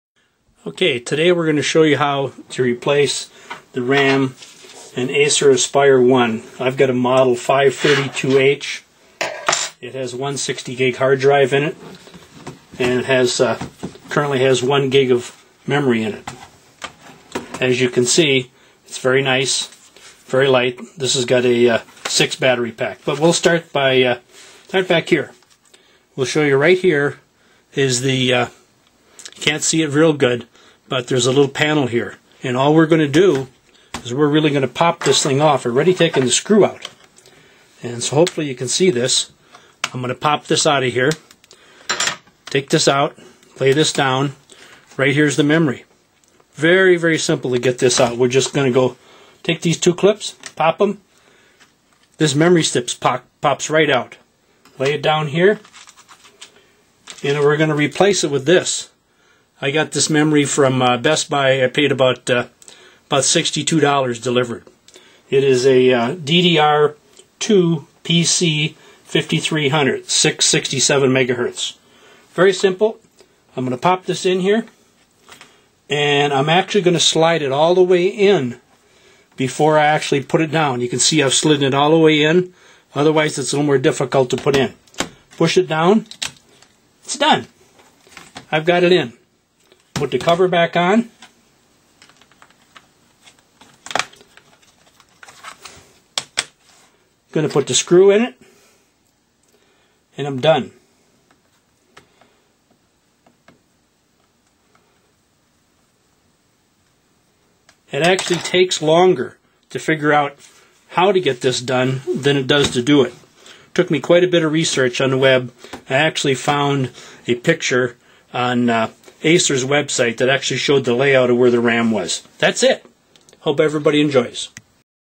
Memory Upgrade Acer Aspire One Model 532h-2268
COMPUTER DIED IN 8 MONTHS!! Upgrading memory for an Acer Aspire One Mode 532h-2268 to 2 GB from 1 BG, using  a single Notebook DDR2 PC2-5300 RAM stick.